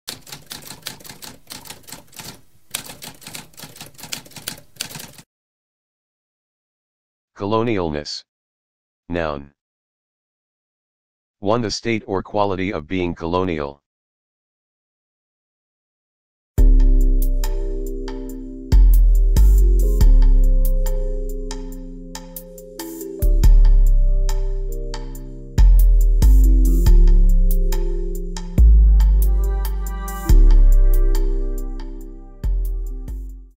What does colonialness mean?
A spoken definition of colonialness.

Intro Sound:
Typewriter - Tamskp